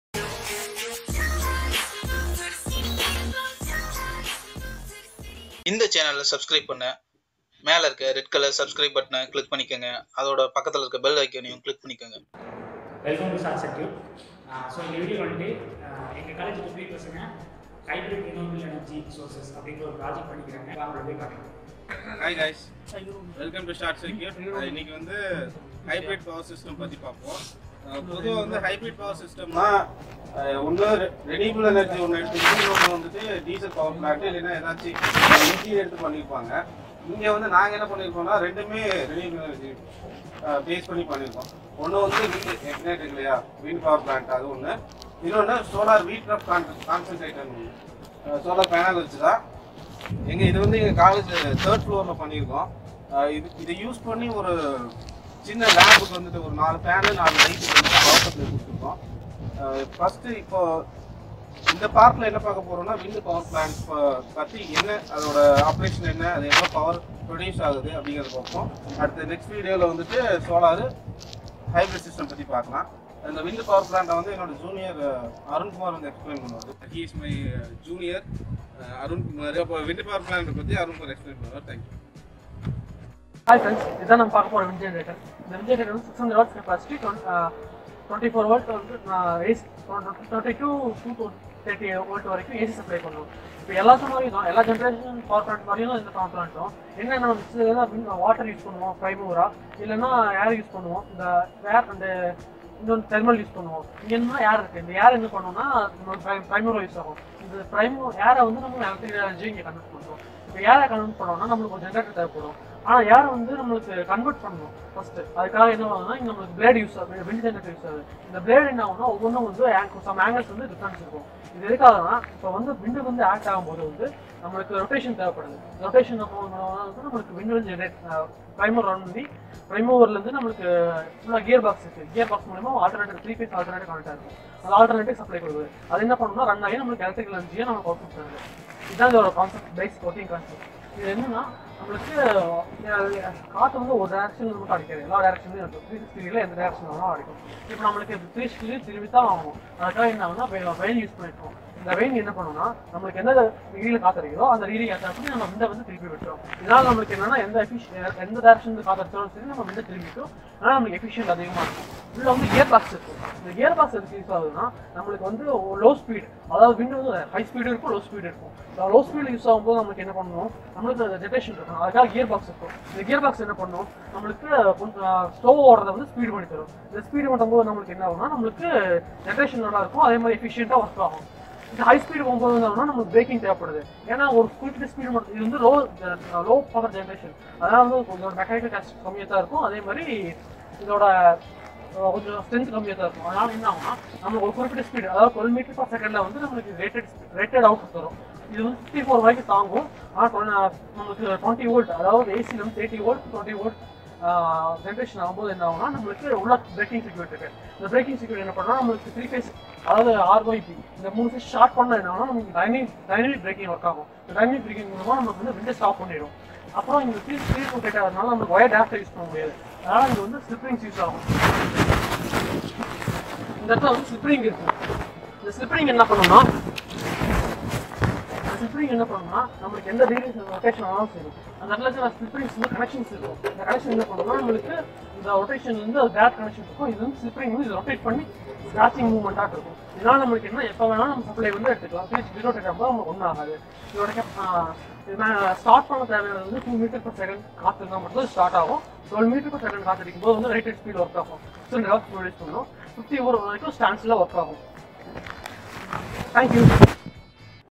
Hybrid system (Wind power plant) part 1 tamil
this video explains hybrid wind power plant which is planted in our college

0:00 / 7:31

Suggested: Why we can't store AC in batteries (tamil)
0:06 / 9:46